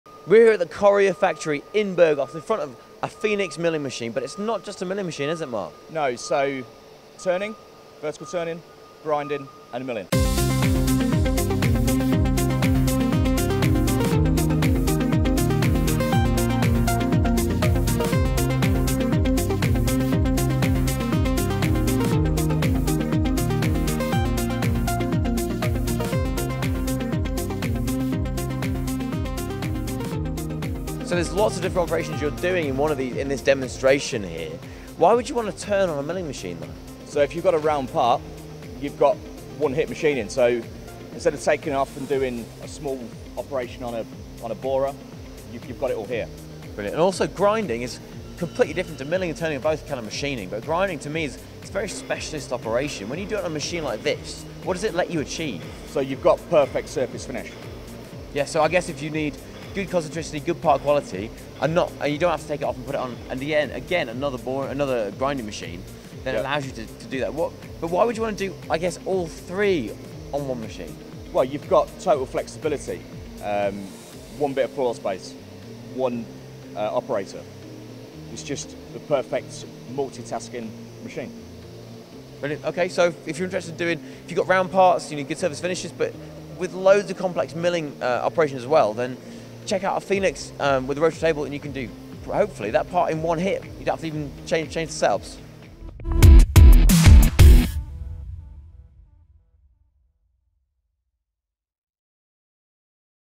Did you know you can grind on these machines?
Multi tasking is becoming popular across machine shops around the World especially on larger and complex parts. The basic principle of installing a multi tasking machine is to eliminate handling time and reduce risk. On display here Rowan speaks to Mark Hone about a Correa Fenix machine that not only mills, it turns and now it grinds as well!!